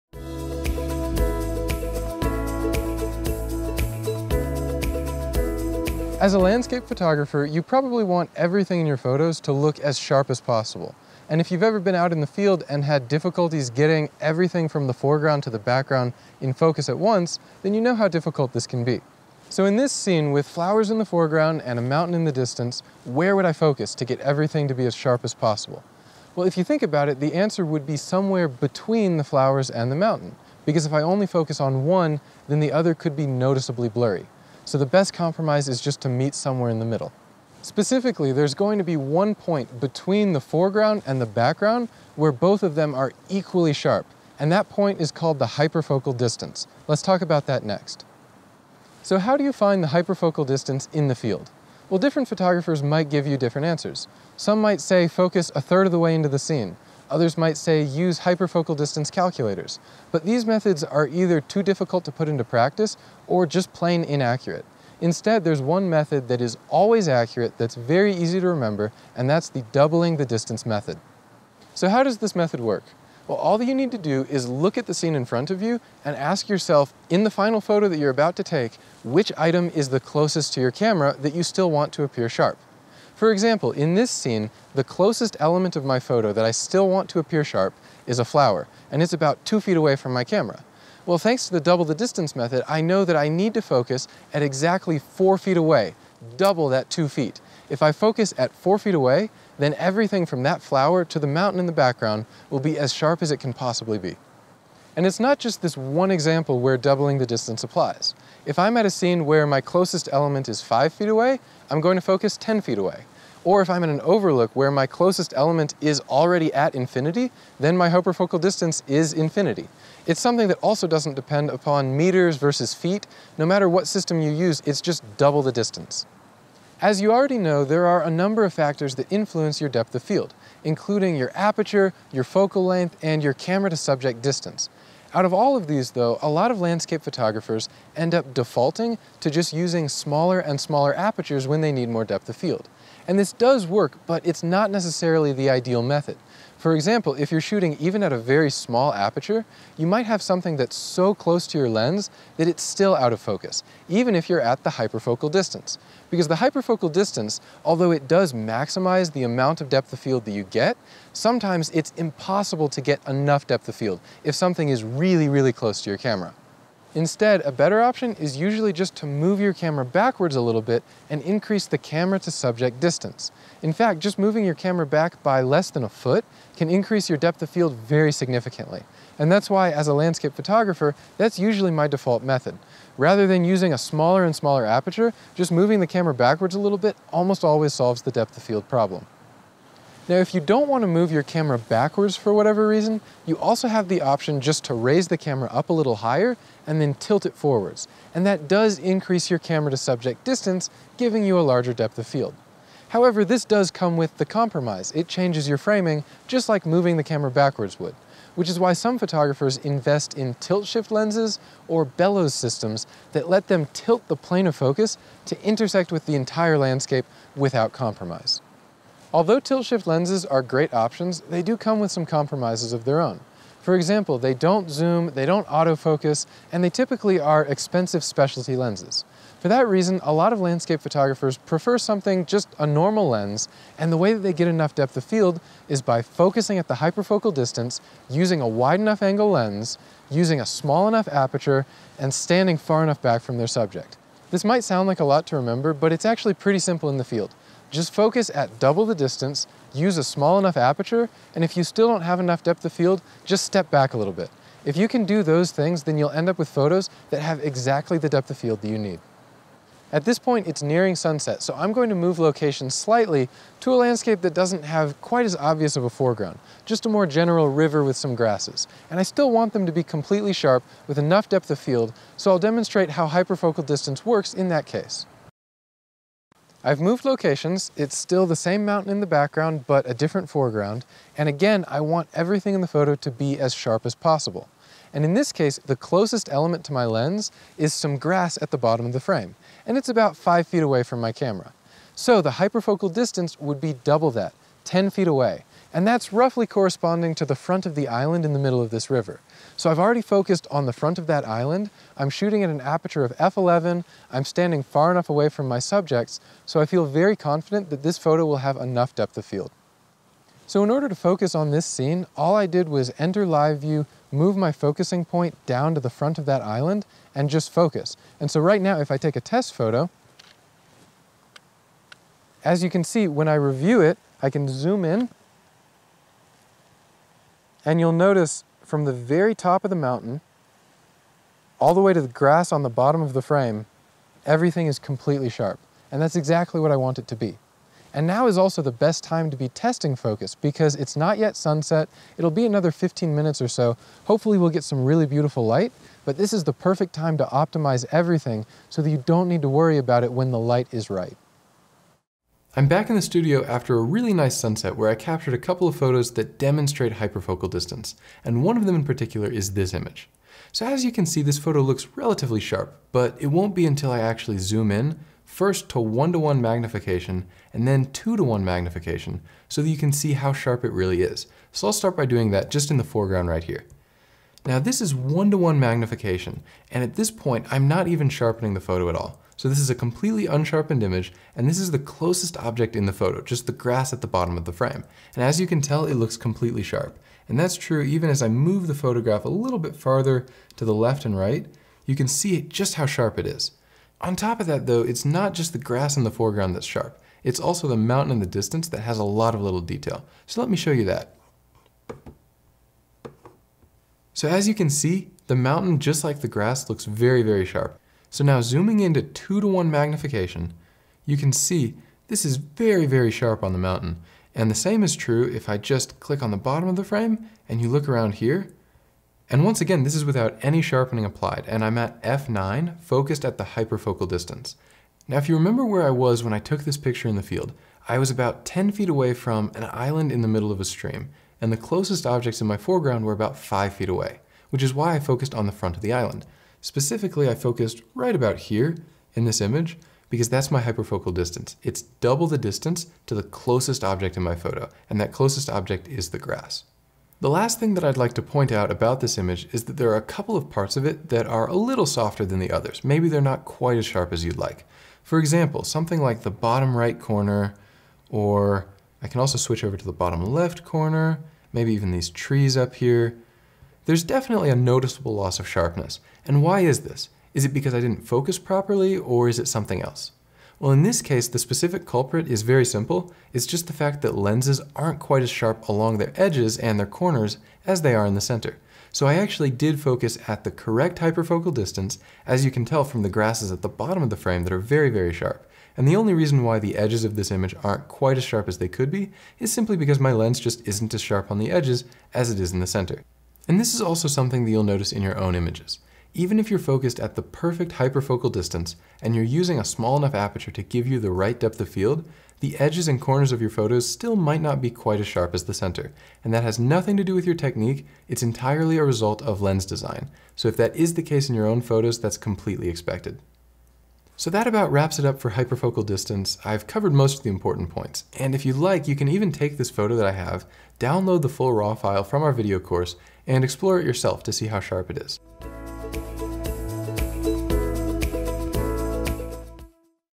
Hyperfocal Distance - Double The Distance Method Explained
In this video, we go over Hyperfocal Distance, how it works and how a simple "Double-the-Distance Method" can simplify the process of focusing in the field in order to get everything in your scene as sharp as possible.

Here is all the photo equipment I use

And here’s my video gear to film this video